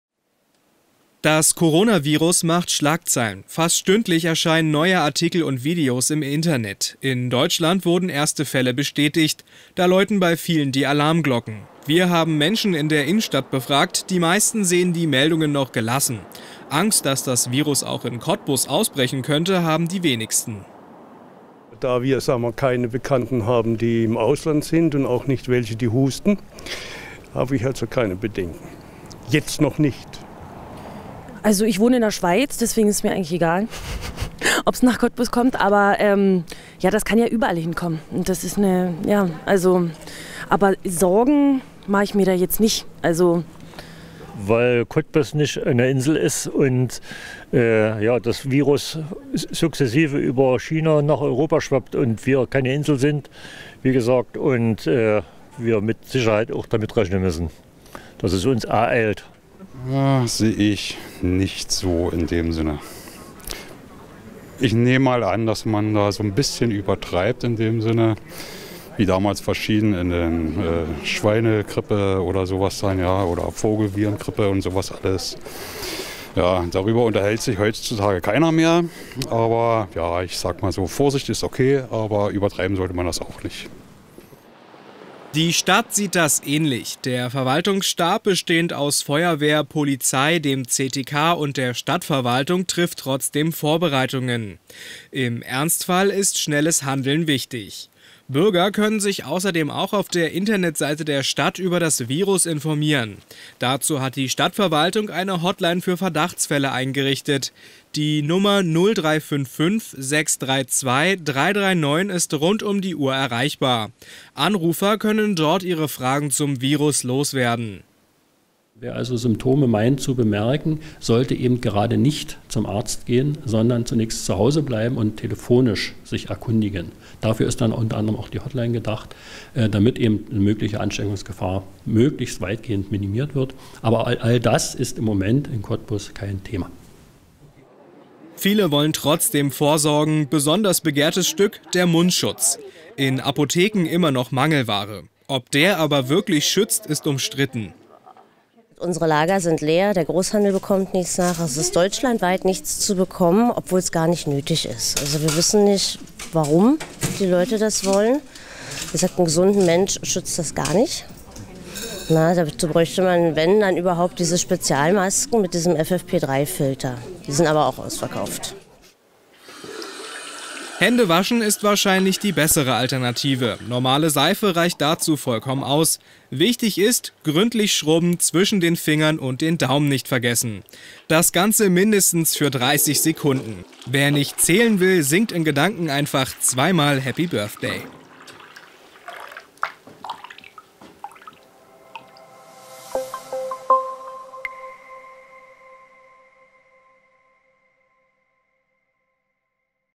Coronavirus auf dem Weg nach Cottbus
Das Virus ist in Deutschland bereits angekommen. Es könnte sich auch in Cottbus ausbreiten. Die Bürger sehen das aber noch gelassen. Auch die Stadtverwaltung lässt sich nicht verrückt machen und schmiedet Pläne für den Ernstfall.